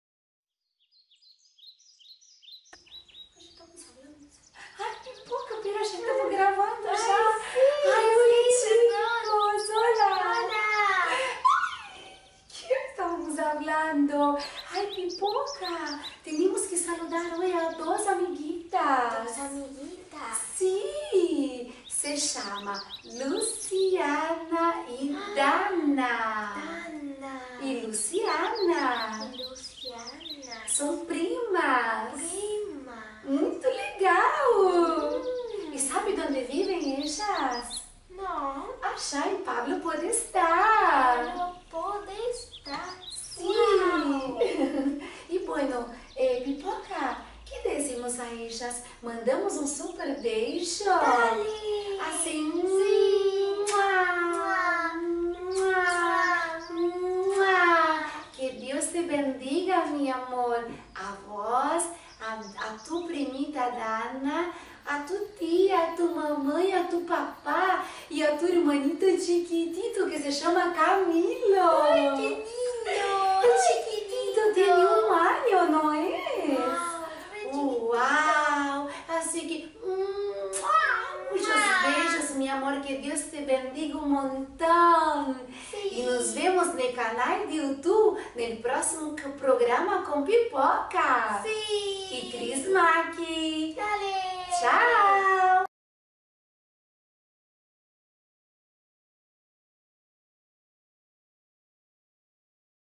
Cris Macki - Saluditos Luciana y Danna de Podestá
Esta sección es para saludar, agradecer a todos los chiquitos que ayudan a crecer a este ministerio.